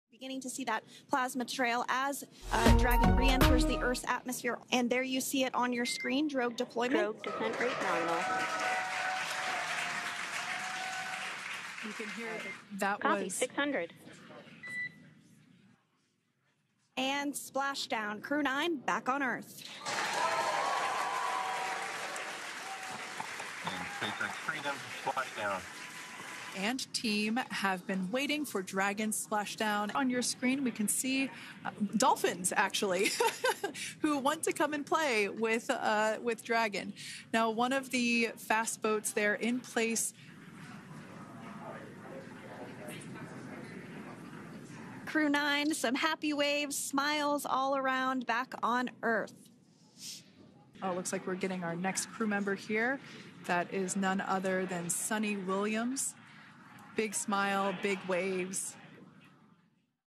NASA-Astronauten landen nach neun Monaten im All wieder auf der Erde
Die NASA-Astronauten Butch Wilmore und Suni Williams sind nach einer neunstündigen Verzögerung aufgrund eines Boeing-Testflugproblems sicher vor der Küste Floridas in einer SpaceX-Kapsel gelandet.